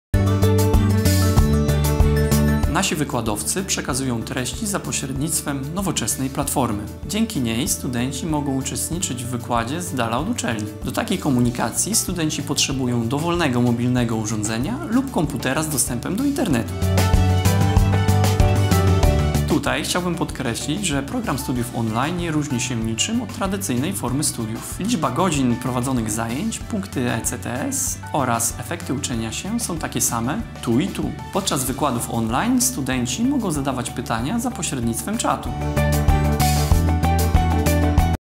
Jak wyglądają studia online?
Jak odbywają się studia online opowiada prodziekan WSB w Gdańsku, dr Miłosz Gac.